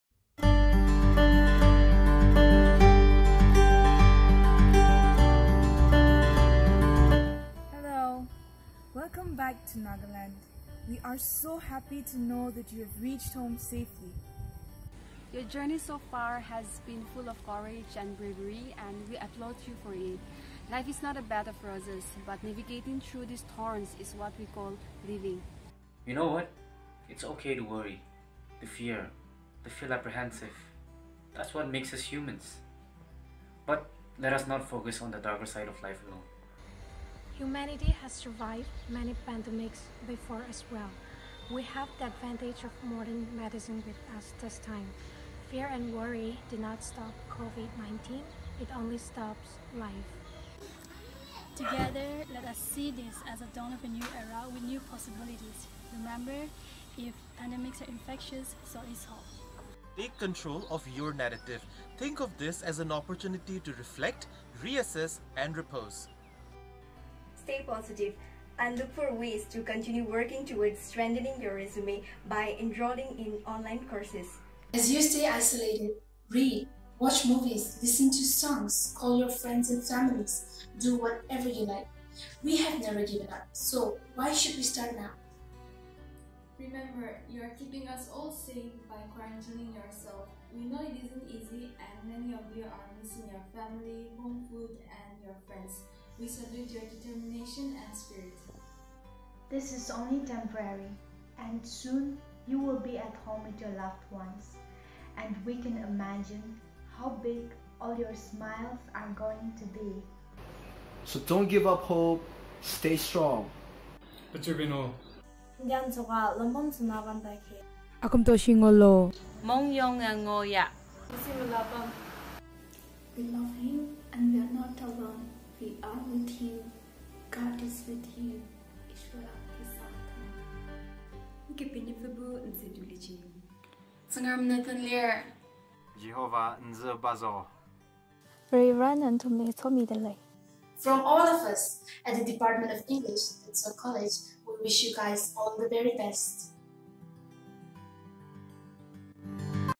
Message to Returnees in Quarantine Center
The Department of English sends out a special message to all the returnees of the various Covid-19 Quarantine Centres across Nagaland